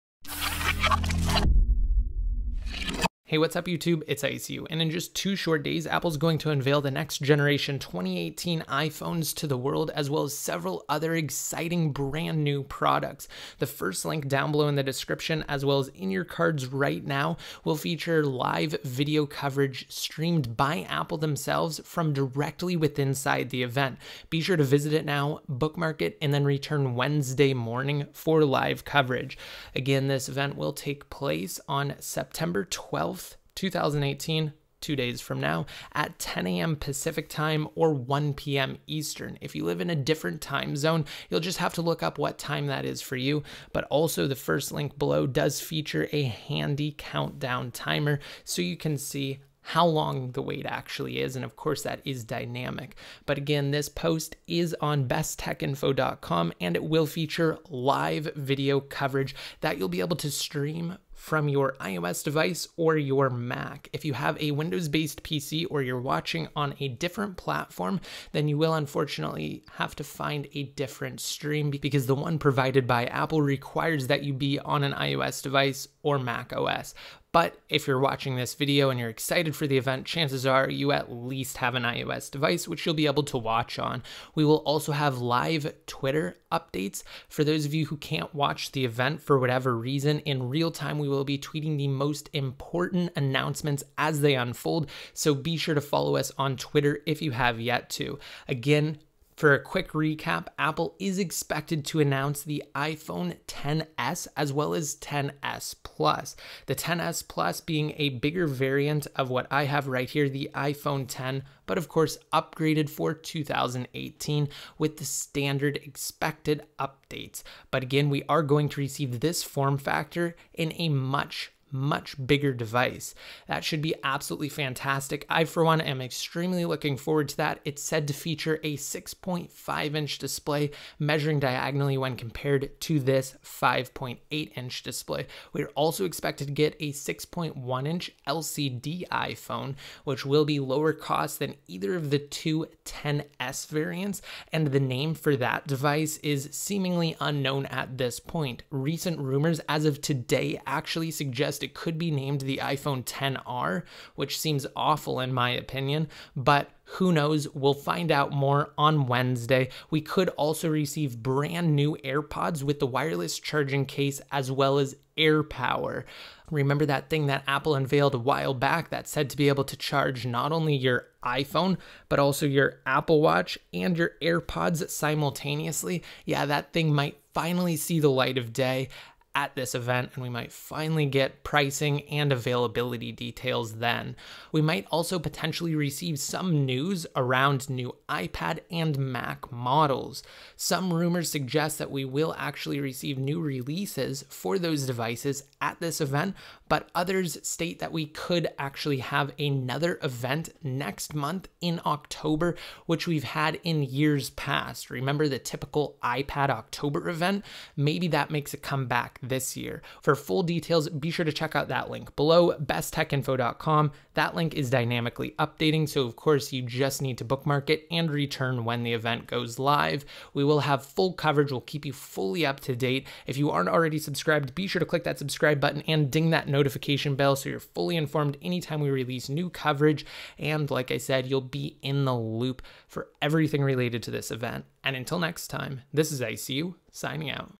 Apple Sept 2018 LIVE - iPhone XS Max: Video Stream Event (iPhone X Plus, 9, iPad X, Apple Watch 4)
Apple iPhone XS Max & iPhone X Plus Event: September 2018 Live - Apple Watch 4, iPad X, iPhone 9 Keynote Unveiling!

Apple’s preparing to unveil the iPhone XS Max, iPhone XS, iPhone 9, Apple Watch Series 4, and AirPods 2.